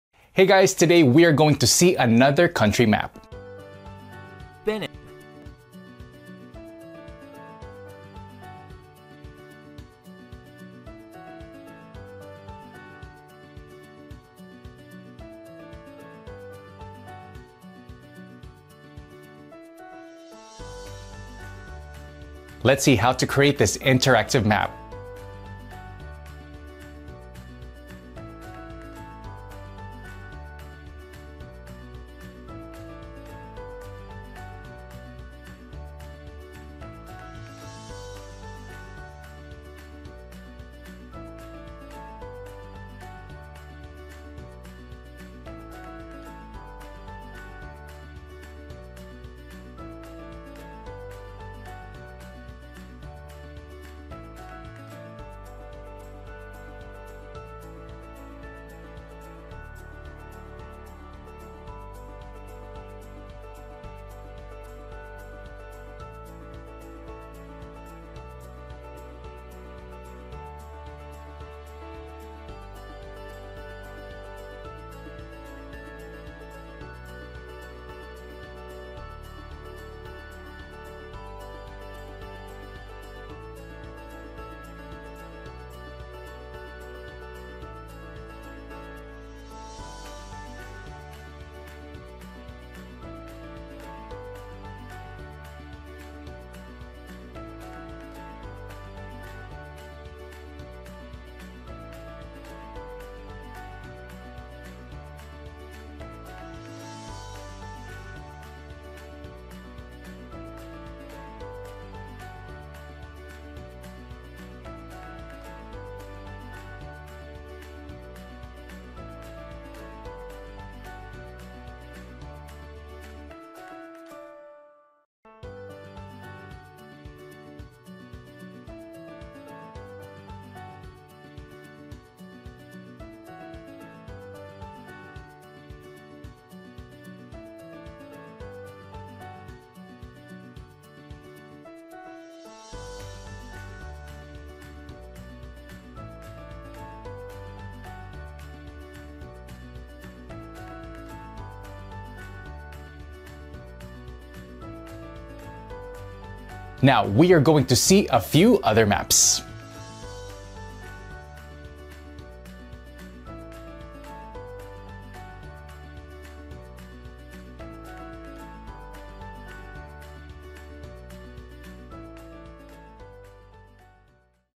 Benin Map – 🌍 Interactive Map of Benin and How to Draw it (Cities and Geography) - SVG Vector
- Interactive Map of Benin: Discover cities and geography of Benin. In this video you will see how to draw the map of Benin and see detailed geography of this country. Let’s discover all the countries in the world with high resolution images.